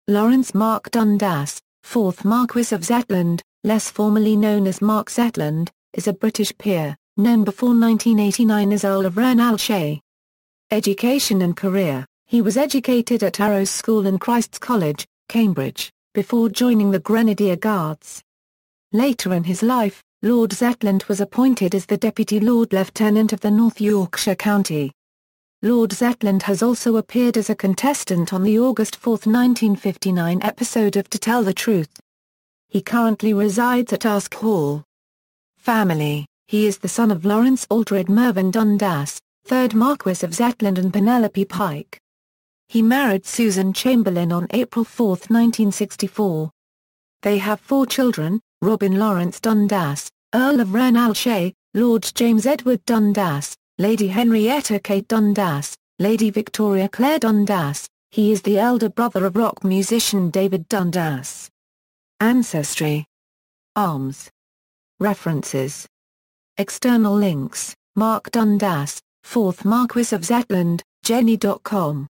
Mark Dundas, 4th Marquess of Zetland
Lawrence Mark Dundas, 4th Marquess of Zetland, less formally known as Mark Zetland, is a British peer, known before 1989 as Earl of Ronaldshay.